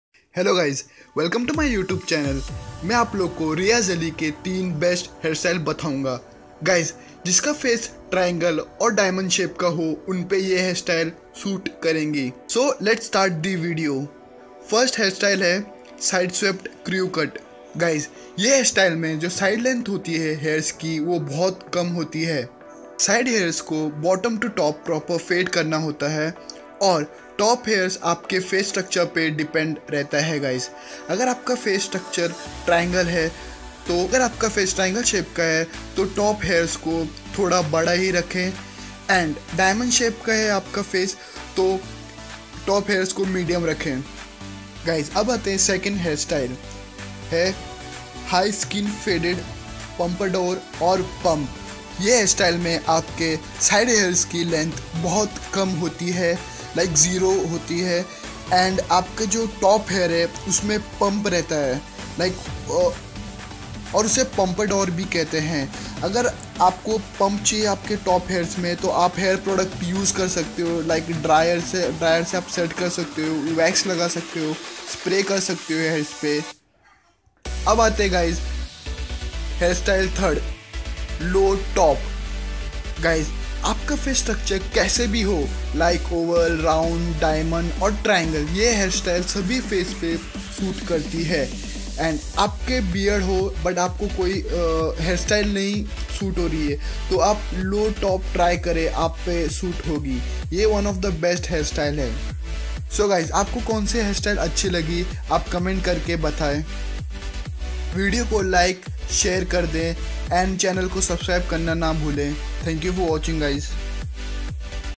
Hairstyles Like Riyaz.14🔥|| New 3 Best Hairstyles of Riyaz Aly.🔥
This video is about- Hairstyles Like Riyaz.14🔥|| New 3 Best Hairstyles of Riyaz Aly.🔥

Guys these are 3 best Hairstyle for you.
As per your face structure you can have these hair style.